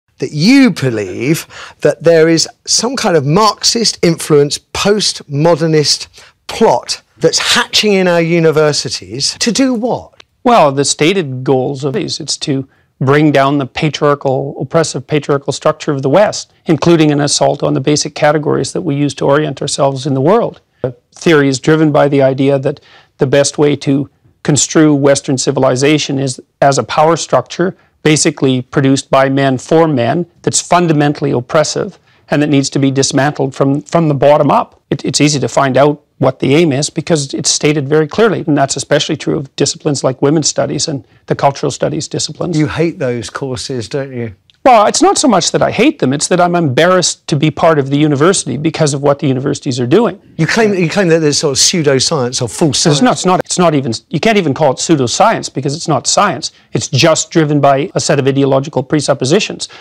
"Those women studies are so bad, i'm embarrassed they exist" - Jordan Peterson
Jordan Peterson on Cathy Newman